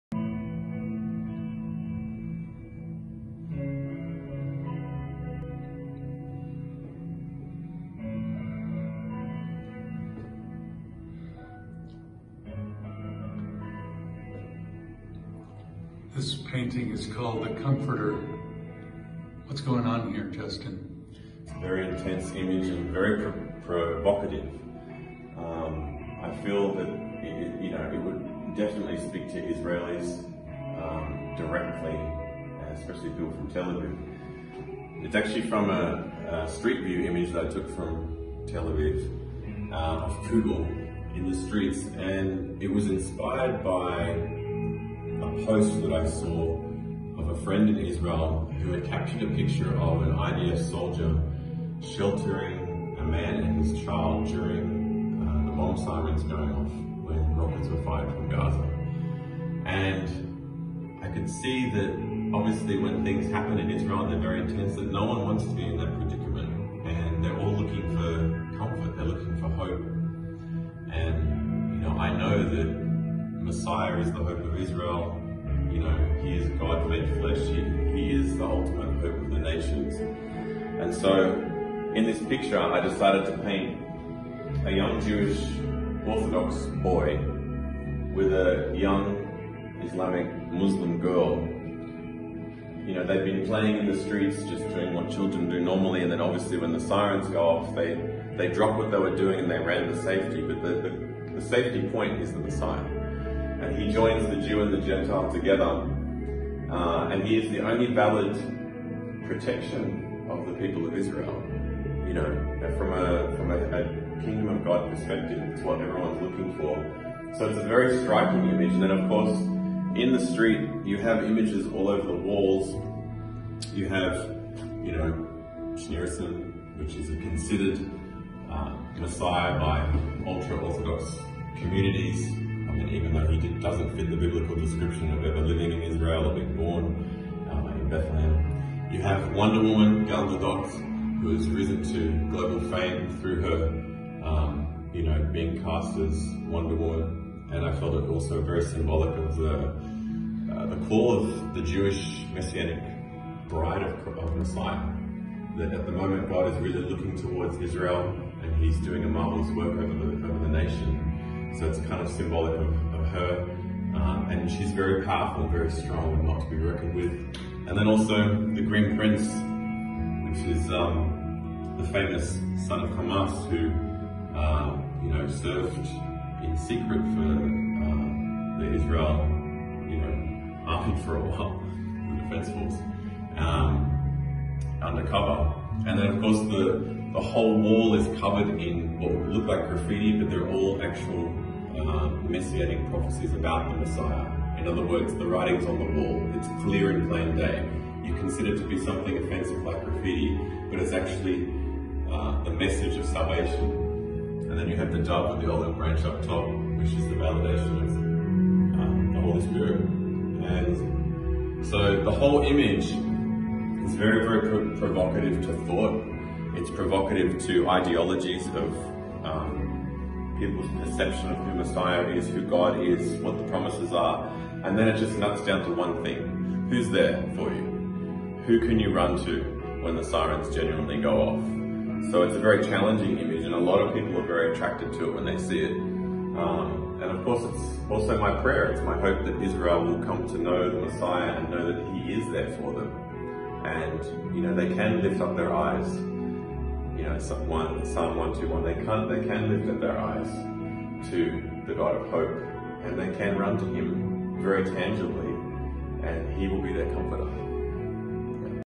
The Comforter:
An in depths tour & explanation of the inspirational yet challenging painting produced to take people deeper in Israel & around the world

included in this month's Gallery installation at The Art lounge Bondi

Be sure to visit in person and see each month's new artist installation.